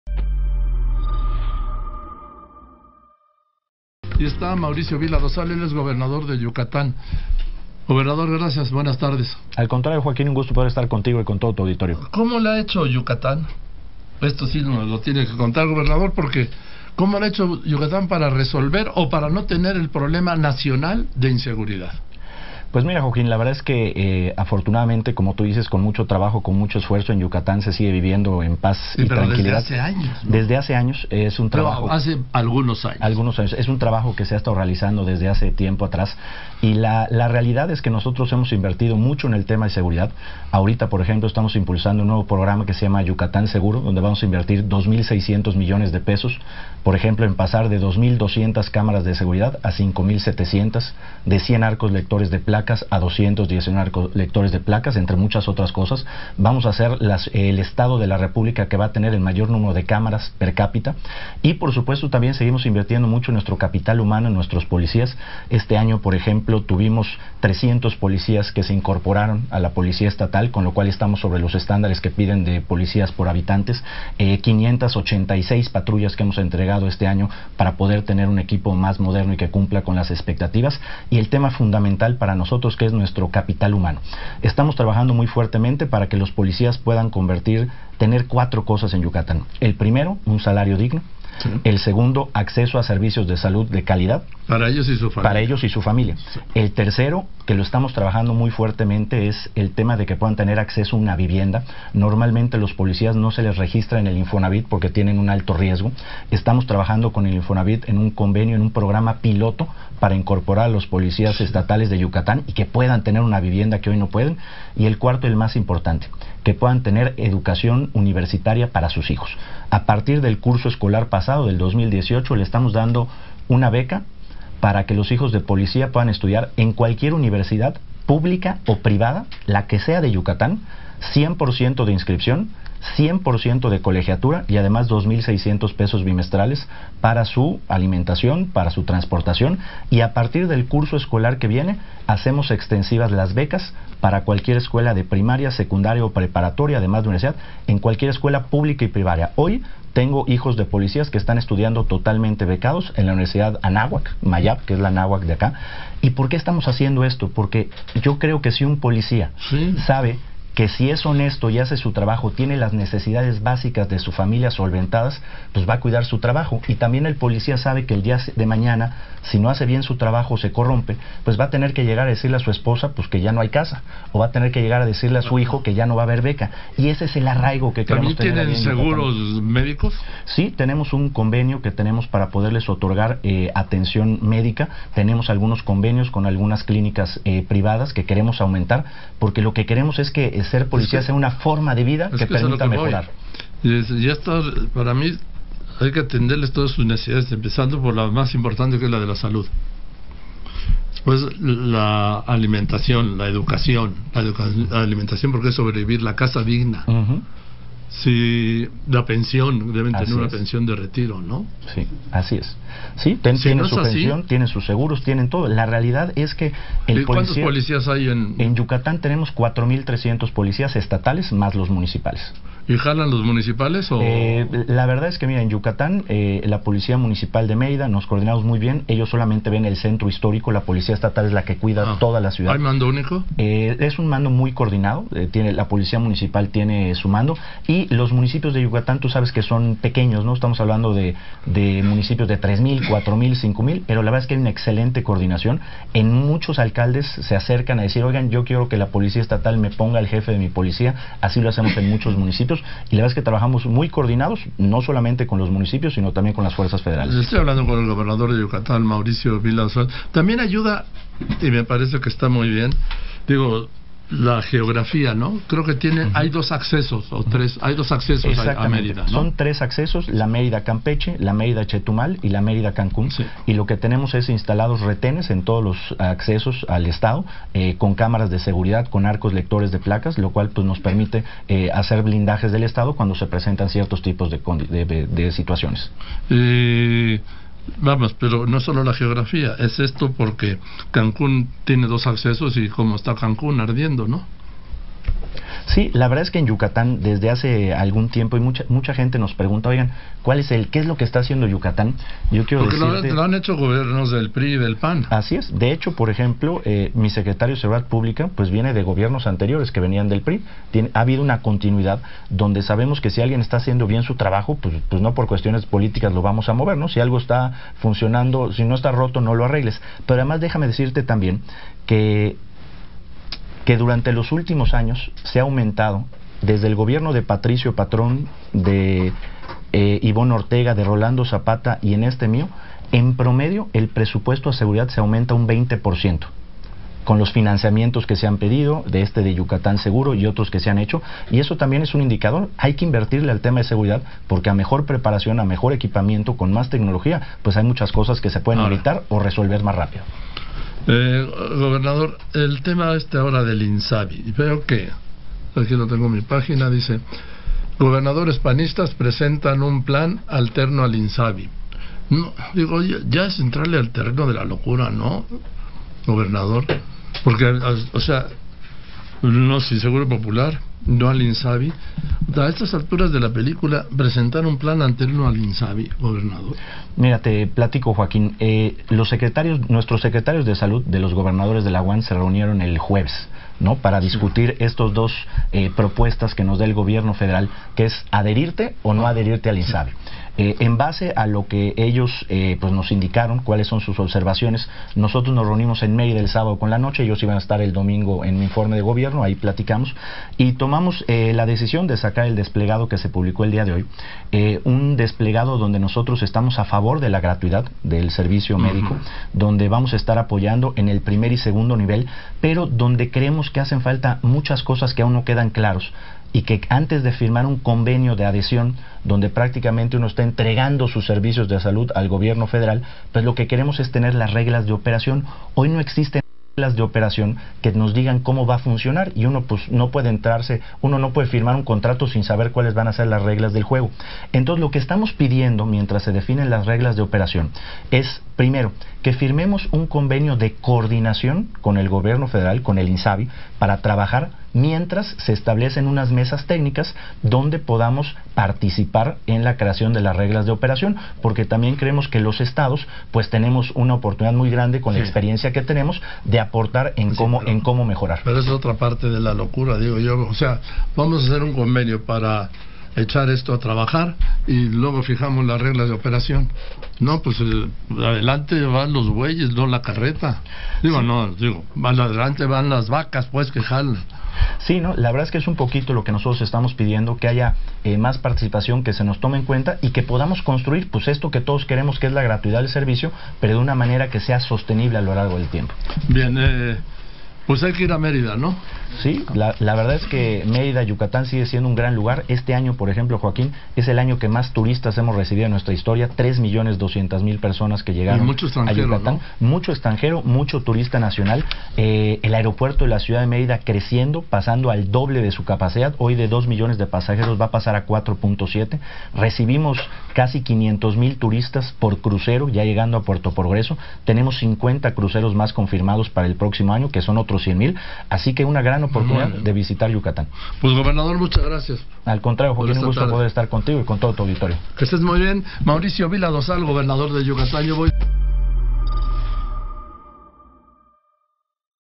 Gobierno de Yucatán invertirá 2 mil 600 mdp en seguridad, asegura Mauricio Vila
Mauricio Vila, gobernador de Yucatán, informó que su administración invertirá 2 mil 600 mdp en seguridad. Aseguró que la prioridad es el capital humano